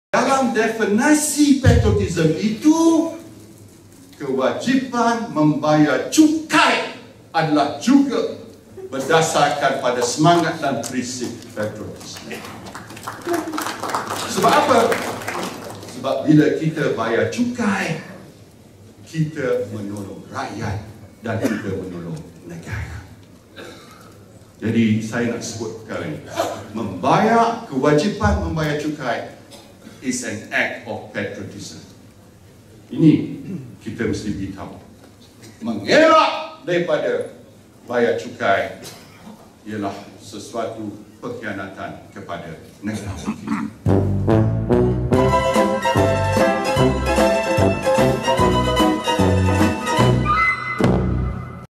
纳吉被追税17亿
来看看
自导自演专家—— 纳吉谈论缴税和逃税～